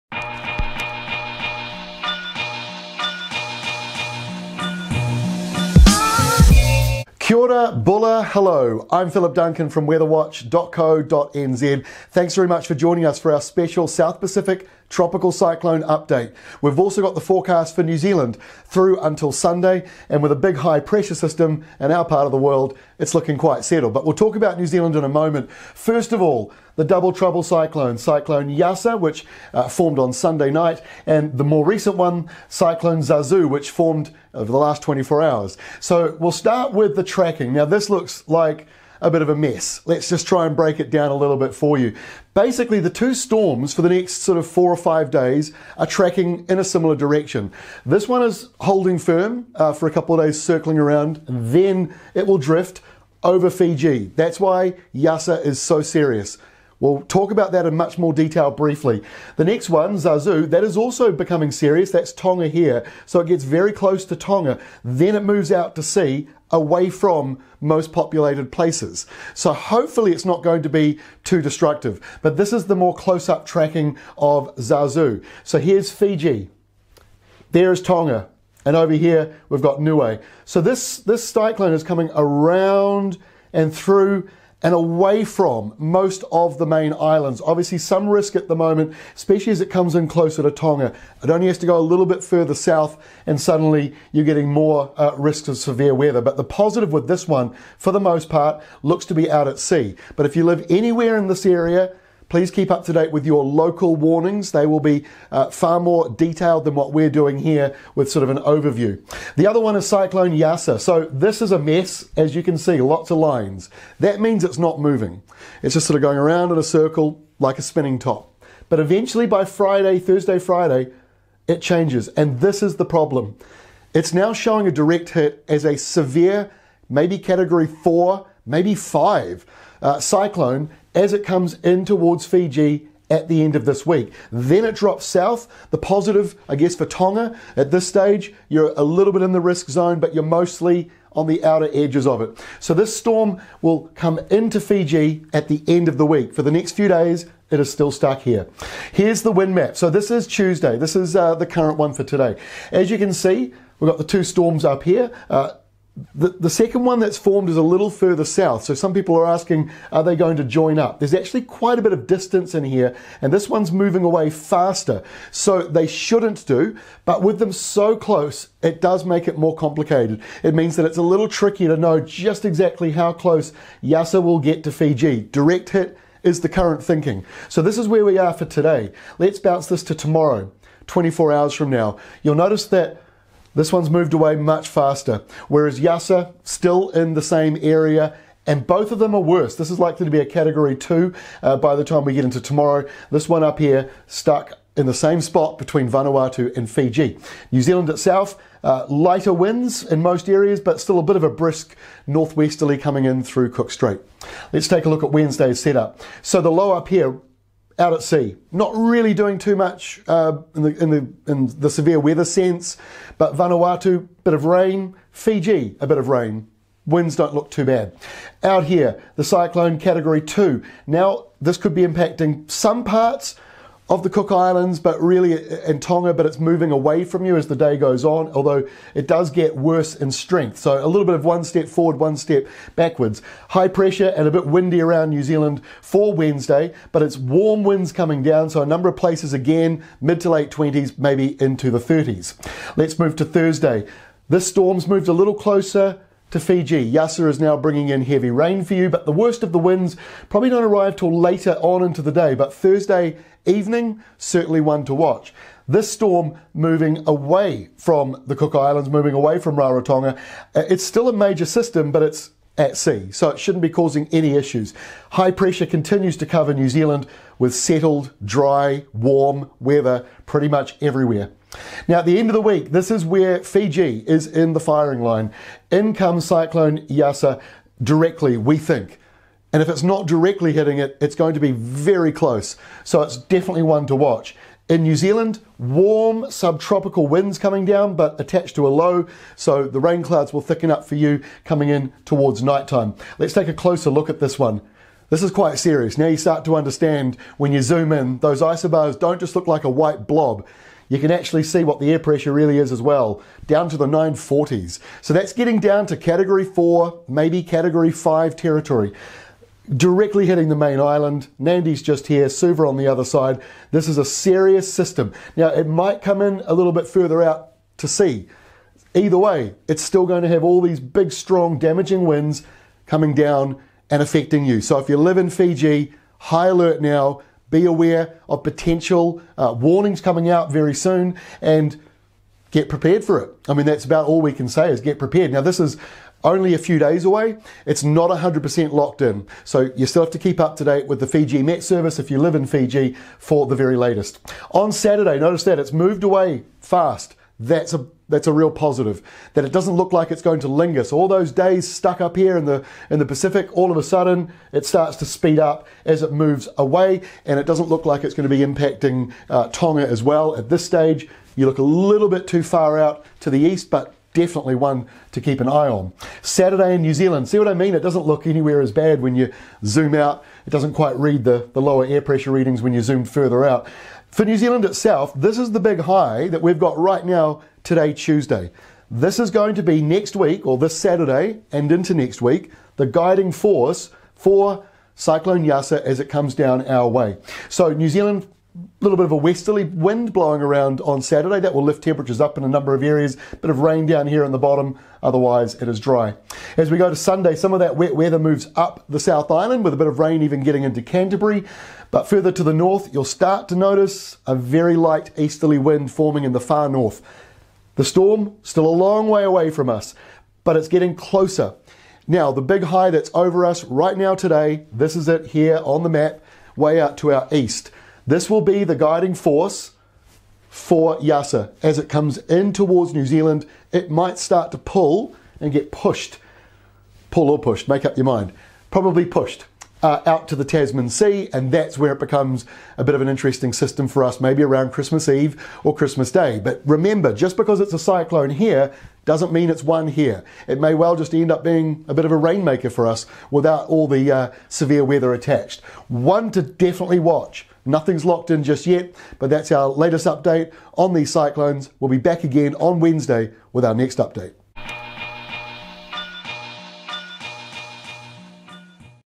Special cyclone update for Fiji, Tonga, NZ
There are two cyclones in the South Pacific now. Cyclone Yasa and Cyclone Zazu.
We have in depth tracking of these two storms - and any possible track towards NZ.
For now, the main focus is Fiji with Cyclone Yasa.
We also have the latest forecast for NZ up until Sunday.